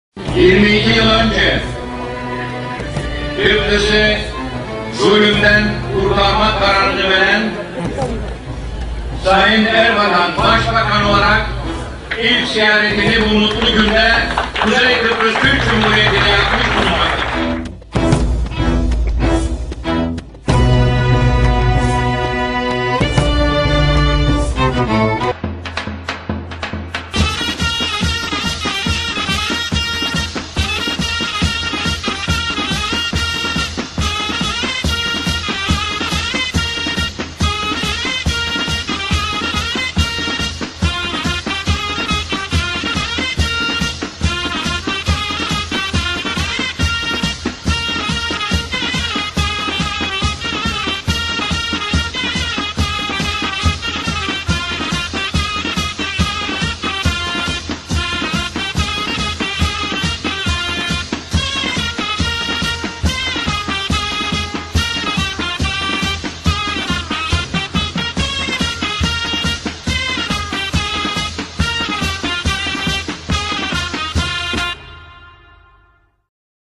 KIBRIS FATİHİ ERBAKAN'I DENKTAŞ BÖYLE TAKDİM ETMİŞTİ....
KIBRIS FATİHİ ECEVİT DEĞİL ERBAKAN'DIR... İŞTE KIBRIS BARIŞ HAREKATININ GERÇEK MİMARI ve İŞTE YAŞANANLAR!.. BİZZAT DENKTAŞ KIBRIS BARIŞ HAREKATI MİMARI OLARAK ERBAKAN'I GÖSTERMESİNE RAĞMEN BU GERÇEĞİ HİÇE SAYAN BEYİNSİZLERE İTHAF OLUNUR!...